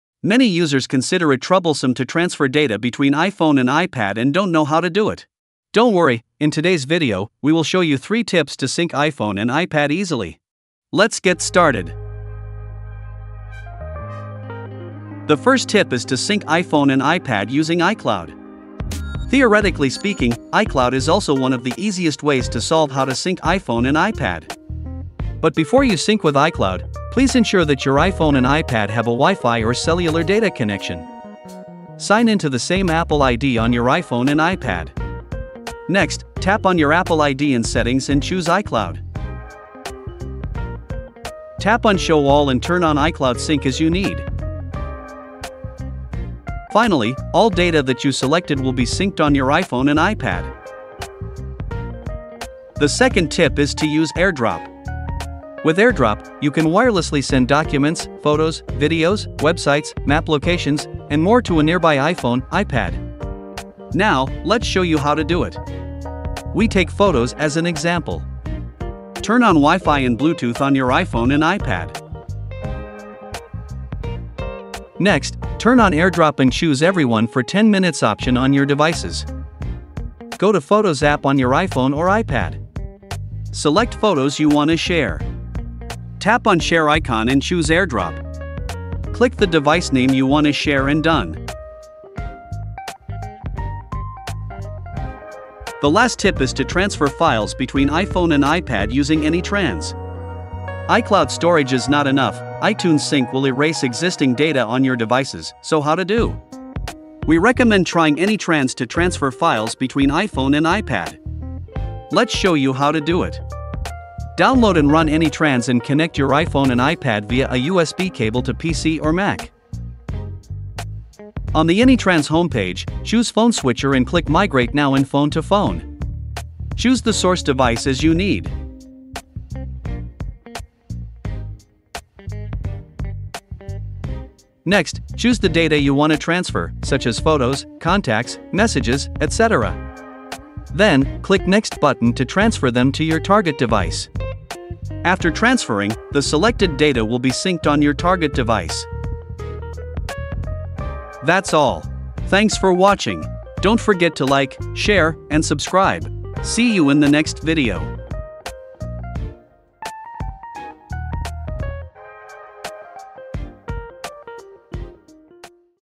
[Top 3 Ways] How to Sync iPhone And iPad | Full Guide
Many users consider it troublesome to transfer data between iPhone and iPad and don’t know how to do it. Don’t worry, in today’s video, we will show you 3 tips to sync iPhone and iPad easily.

Though iPhone and iPad use the same system, they are pretty different. If you wanna know how to sync your important data between iPhone and iPad, please don't miss this video!
00:00 Intro
00:15 Tip 1. Sync iPhone and iPad using iCloud
00:53 Tip 2. Sync iPhone and iPad using AirDrop
01:36 Tip 3. Transfer Files Between iPhone and iPad using AnyTrans